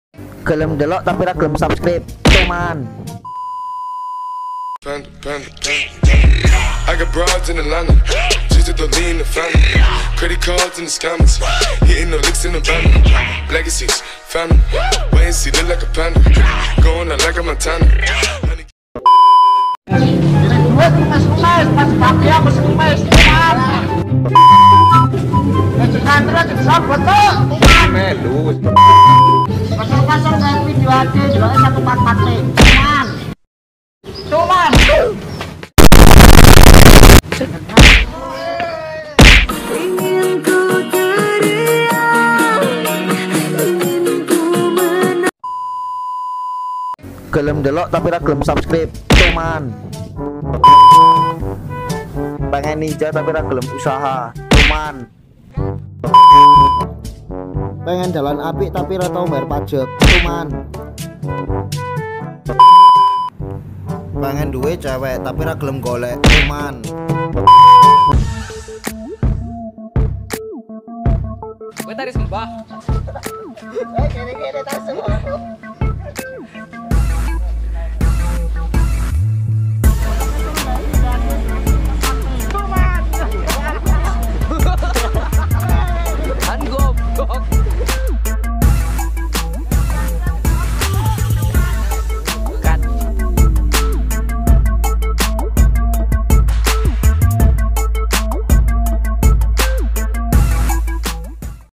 Meme Tuman viral | Tuman Versi MOTOVLOG
Tuman viral | Tuman Versi MOTOVLOG

Maaf jika audio suara saya gak jelas,karna mic action cam lagi ngacok waktu ngerekam audionya gak keluar.
Itu lah video Tuman Versi MOTOVLOG dari saya dan kawan" kencleng saya.

Riziq motovlog

Camera shooting
@Imam fr

Kalo gak suka ya cukup dislike aja atau komen juga bisa agar saya bisa memperbaiki cara belajar saya hehehe.

Oke terima kasih buat yang udah nonton sampek akhir, saya do'ain semoga rejekinya lancar,amin.